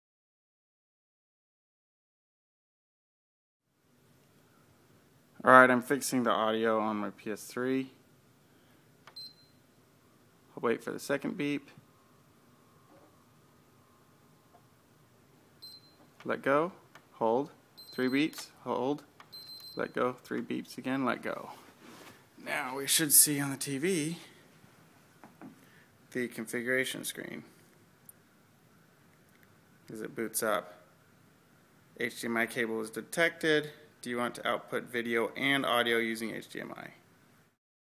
Fix Sound on PS3 Games
How to fix the sound if it stops working on PS3 games.